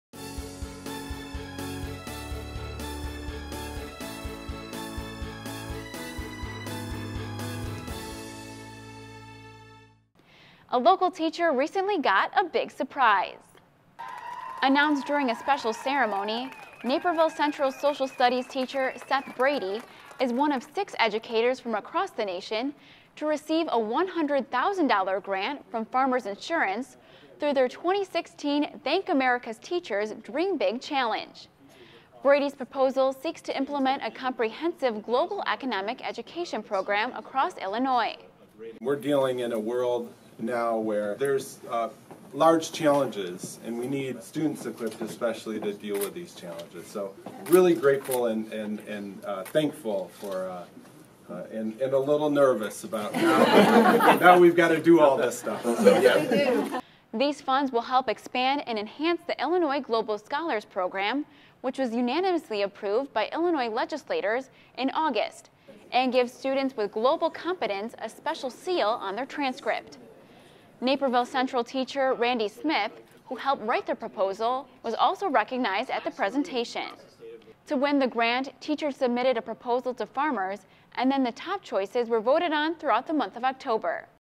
Naperville Central Teacher Gets a Big Grant
Announced during a surprise ceremony, Naperville Central social studies teacher Seth Brady is one of six educators from across the nation to receive a $100,000 grant from Famers Insurance through their 2016 Thank America’s Teachers Dream Big Challenge.

Brady’s proposal seeks to implement a comprehensive global economic education program across Illinois.

“We’re dealing in a world now where there’s large challenges and we need students especially, who are equipped to deal with these challenges. So really grateful, and thankful for and a little nervous about now, now we’ve got to do all this stuff,” Brady said.

These funds will help expand and enhance the Illinois Global Scholars program, which was unanimously approved by Illinois legislators in August and gives students with global competence a special seal on their transcript.

Naperville Central teacher, Randy Smith, who helped write the proposal was also recognized at the presentation.

To win the grant, teachers submitted a proposal to Famers, and then the top choices were voted on throughout the month of October.

Naperville News 17’s Evan Summers reports.